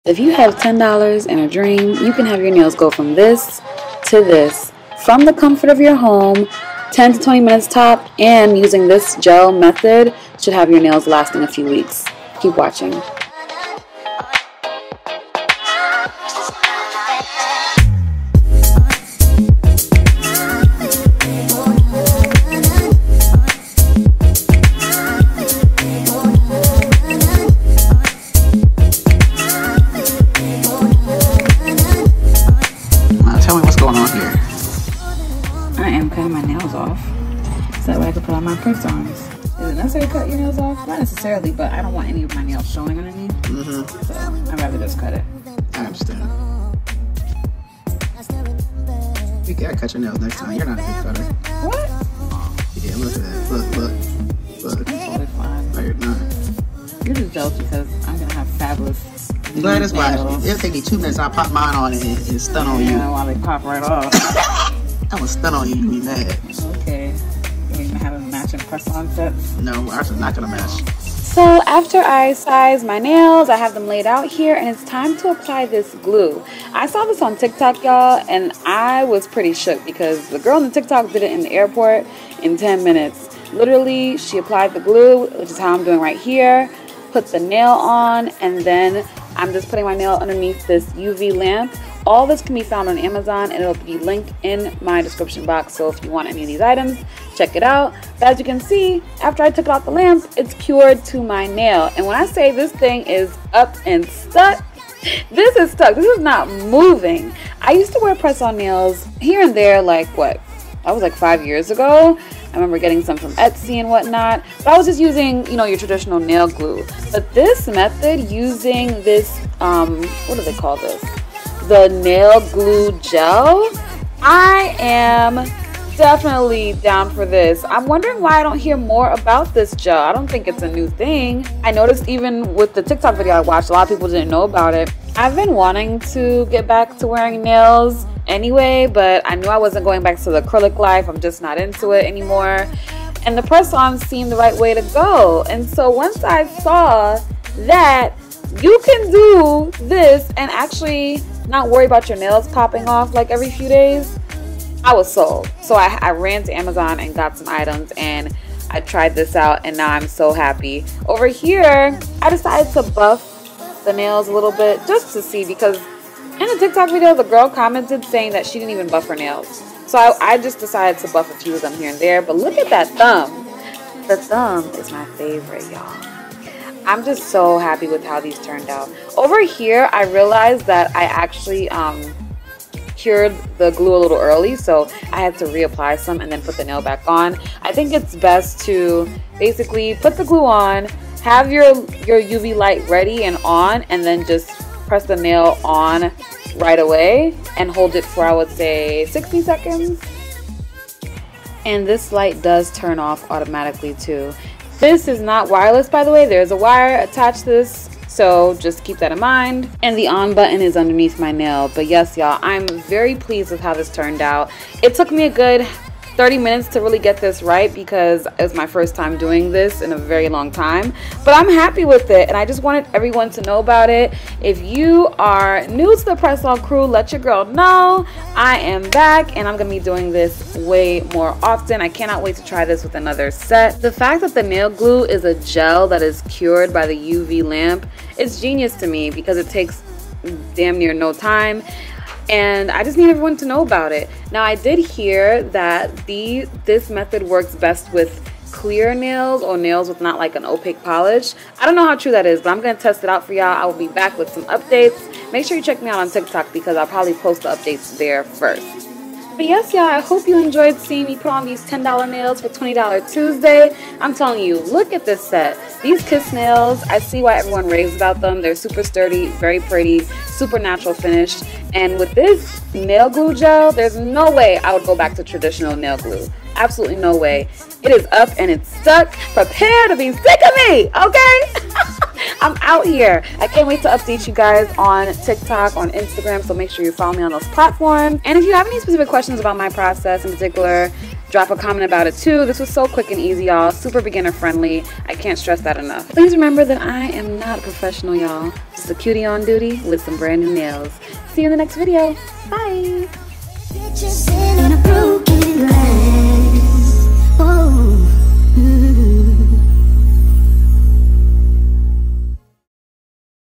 $7 PRESS ONS CAN LAST! UV Nail Glue Gel Method! | $20 Tues, Ep. 80
This UV GEL NAIL GLUE method just changed the game for me!

N A I L   P R O D U C T S   U S E D  ✨

F I L M   E Q U I P M E N T  🎥
▷ Macbook Air
▷ Camera : CANON M6 Mark ii
▷ Lens : SIGMA 30mm 1.4

Love Always,
Gwladys